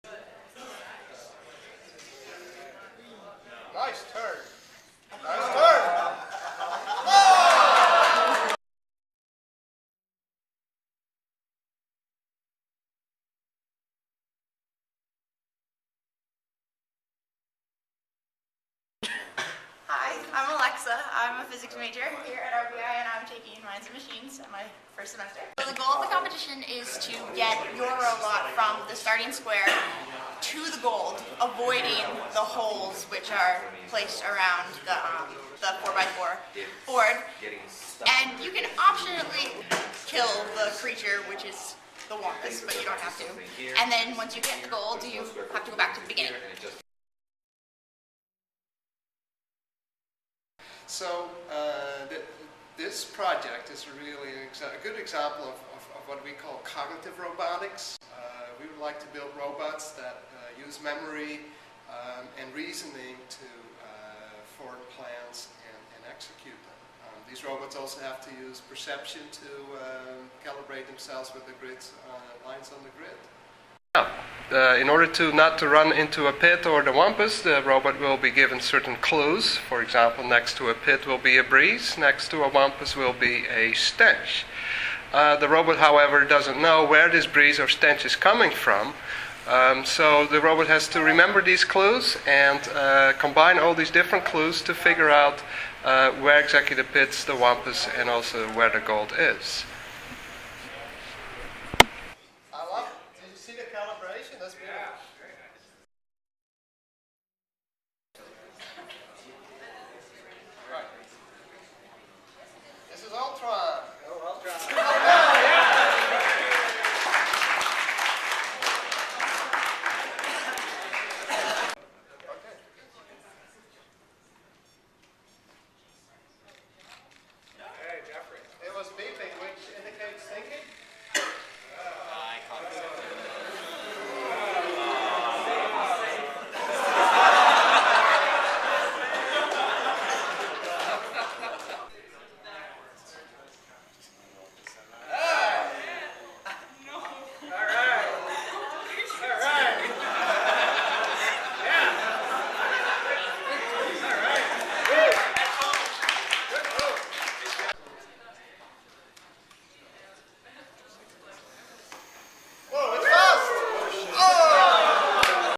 2011 Wumpus Competition
The final project for students in the freshman Minds & Machines class at Rensselaer is to build and program a LEGO robot to solve the "Wumpus World Problem", requiring students to think about perception, action, reasoning, and decision making. It is a non-trivial problem, and each year only few teams are successful!